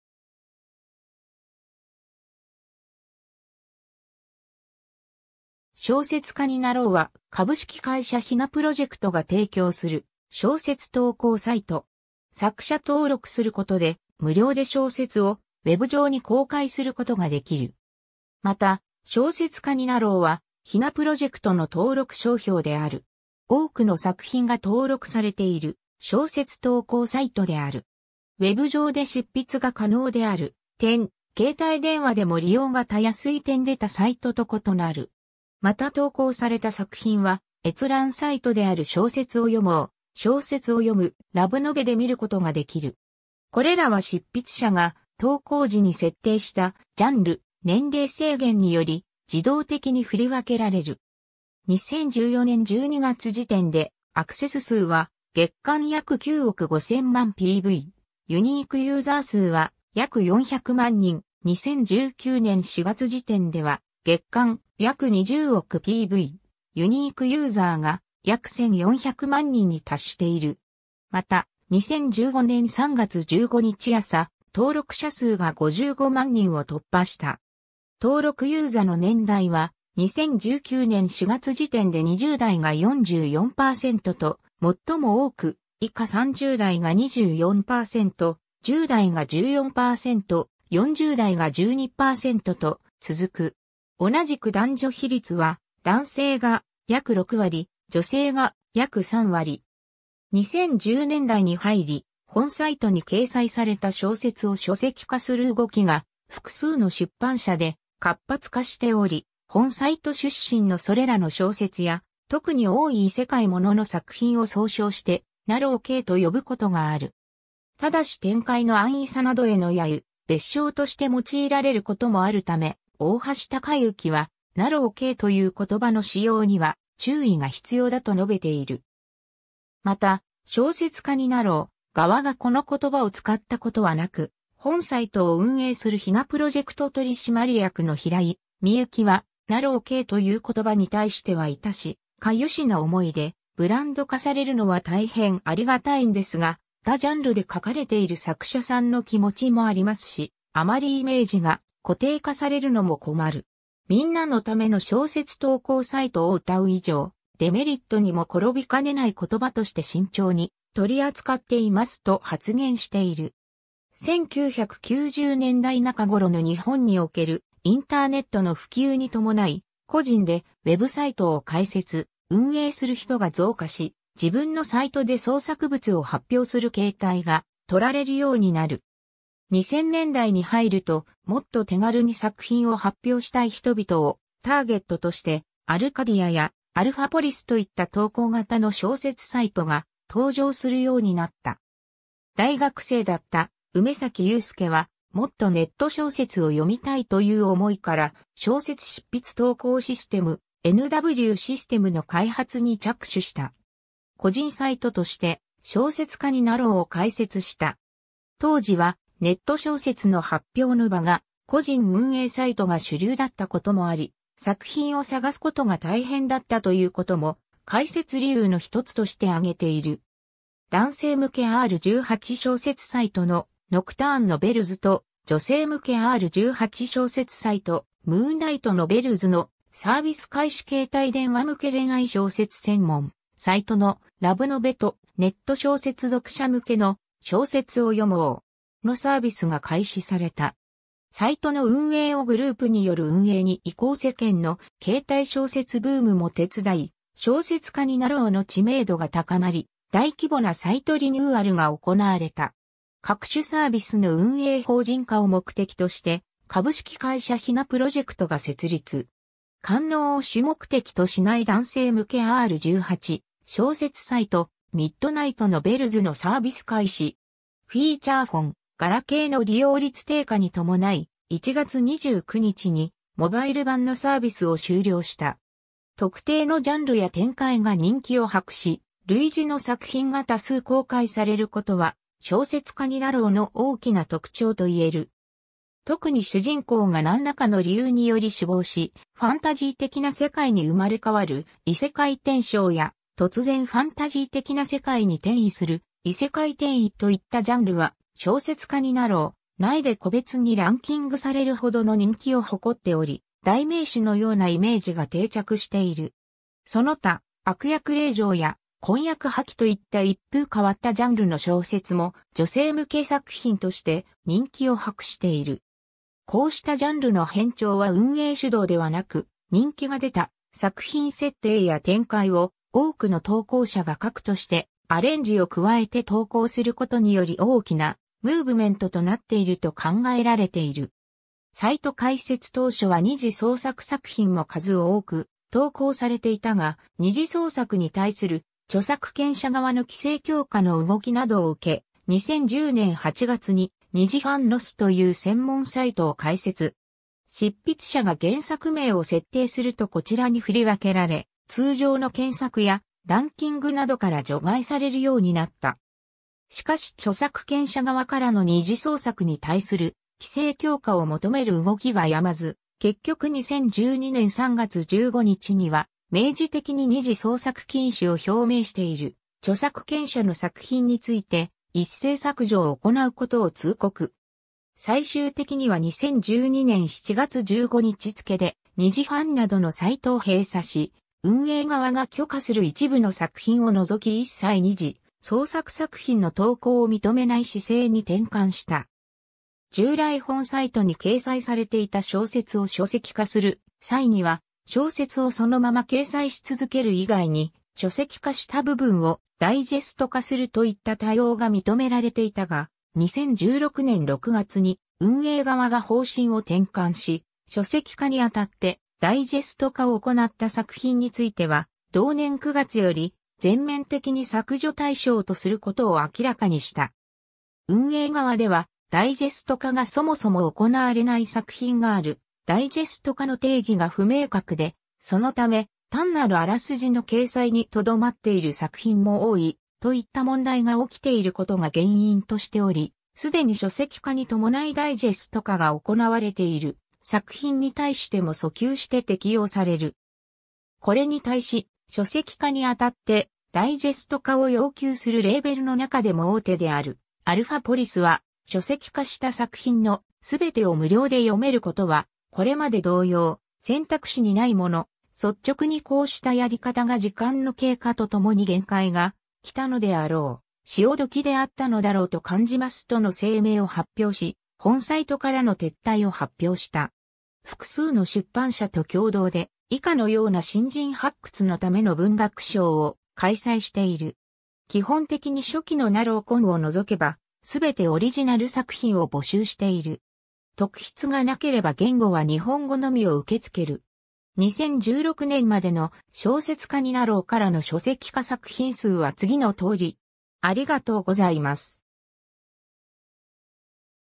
小説家になろう
小説家になろうは、株式会社ヒナプロジェクトが提供する小説投稿サイト。
作者登録することで、無料で小説をウェブ上に公開することができる。
また「小説家になろう」はヒナプロジェクトの登録商標である。
 多くの作品が登録されている小説投稿サイトである。
ウェブ上で執筆が可能である点、携帯電話でも利用（執筆や閲覧）が容易い点で他サイトと異なる。
また投稿された作品は、閲覧サイトである小説を読もう（一般向け作品、PC向け）・小説を読む（一般向け作品、携帯向け）・ラブノベ（恋愛小説、携帯向け）で見ることができる。
これらは執筆者が投稿時に設定したジャンル・年齢制限により自動的に振り分けられる。
 2014年12月時点でアクセス数は月間約9億5000万PV、ユニークユーザー数は約400万人、2019年4月時点では月間約20億PV、ユニークユーザーが約1400万人に達している。
また、2015年3月15日朝、登録者数が55万人を突破した。
登録ユーザの年代は、2019年4月時点で20代が44%と最も多く、以下30代が24%、10代が14%、40代が12%と続く。
同じく男女比率は、男性が約6割、女性が約3割（残りは性別未登録）。
 2010年代に入り、本サイトに掲載された小説を（多くは加筆・修正を経て）書籍化する動きが複数の出版社で活発化しており、本サイト出身のそれらの小説や、特に多い異世界ものの作品を総称して「なろう系」と呼ぶことがある。
ただし展開の安易さなどへの揶揄、蔑称として用いられることもあるため、大橋崇行は「なろう系」という言葉の使用には注意が必要だと述べている。
また「小説家になろう」側がこの言葉を使ったことはなく、本サイトを運営するヒナプロジェクト取締役の平井幸は「なろう系」という言葉に対しては痛し痒しな思いで「ブランド化されるのは大変ありがたいんですが、他ジャンルで書かれている作者さんの気持ちもありますし、あまりイメージが固定化されるのも困る。『みんなのための小説投稿サイト』を謳う以上、デメリットにも転びかねない言葉として慎重に取り扱っています」と発言している。
 1990年代中ごろの日本におけるインターネットの普及に伴い、個人でWebサイトを開設・運営する人が増加し、自分のサイトで創作物を発表する形態が取られるようになる。
2000年代に入るともっと手軽に作品を発表したい人々をターゲットとしてアルカディアやアルファポリスといった投稿型の小説サイトが登場するようになった。
 大学生だった梅崎祐輔は「もっとネット小説を読みたい」という思いから小説執筆投稿システム「NW-SYSTEM」の開発に着手した。
 個人サイトとして「小説家になろう」を開設した。
当時はネット小説の発表の場が個人運営サイトが主流だったこともあり、作品を探すことが大変だったということも開設理由のひとつとして挙げている。
 男性向けR18小説サイトの「ノクターンノベルズ」と女性向けR18小説サイト「ムーンライトノベルズ」のサービス開始 携帯電話向け恋愛小説専門サイトの「ラブノベ」とネット小説読者向けの「小説を読もう！」のサービスが開始された。
 サイトの運営をグループによる運営に移行 世間のケータイ小説ブームも手伝い、「小説家になろう」の知名度が高まり、大規模なサイトリニューアルが行われた。
 各種サービスの運営法人化を目的として株式会社ヒナプロジェクトが設立。
 (「ノクターンノベルズ」「ムーンライトノベルズ」「ミッドナイトノベルズ」をヒナプロジェクトの関連会社であるナイトランタンへ運営移管) 官能を主目的としない男性向けR18小説サイト「ミッドナイトノベルズ」のサービス開始。
 フィーチャーフォン/ガラケーの利用率低下に伴い、1月29日にモバイル版のサービスを終了した。
 特定のジャンルや展開が人気を博し、類似の作品が多数公開されることは「小説家になろう」の大きな特徴と言える。
特に主人公がなんらかの理由により死亡し、ファンタジー的な世界に生まれ変わる「異世界転生」や突然ファンタジー的な世界に転移する「異世界転移」といったジャンルは「小説家になろう」内で個別にランキングされるほどの人気を誇っており、代名詞のようなイメージが定着している。
その他、「悪役令嬢」や「婚約破棄」といった一風変わったジャンルの小説も女性向け作品として人気を博している。
こ...